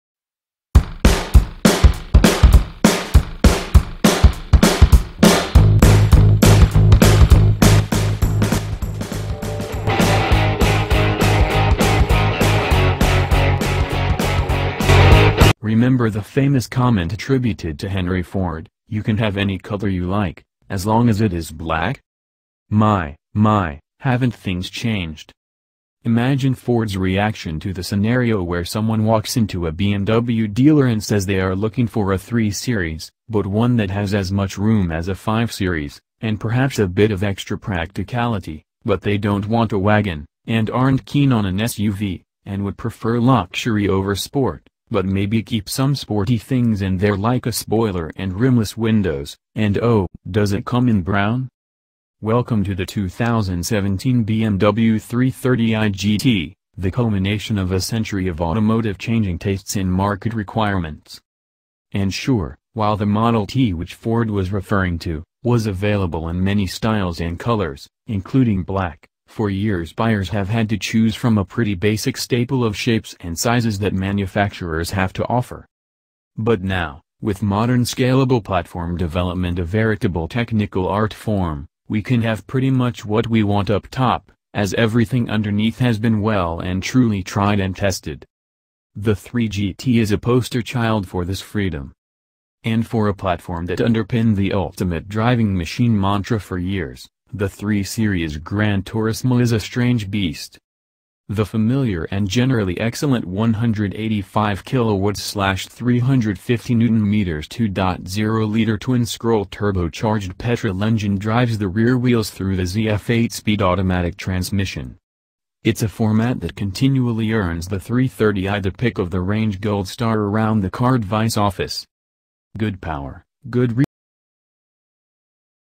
2017 BMW 330i GT | Review Interior & Exterior
It's a 3 that's as long as a 5 with as much room as an X, but is the 330i Gran Turismo BMW enough?
Remember the famous comment attributed to Henry Ford, “you can have any colour you like, as long as it is black”?

My, my, haven’t things changed.

Imagine Ford’s reaction to the scenario where someone walks into a BMW dealer and says they are looking for a 3 Series, but one that has as much room as a 5 Series, and perhaps a bit of extra practicality, but they don’t want a wagon, and aren’t keen on an SUV, and would prefer luxury over sport, but maybe keep some sporty things in there like a spoiler and rimless windows, and oh, does it come in brown?

Welcome to the 2017 BMW 330i GT, the culmination of a century of automotive changing tastes and market requirements.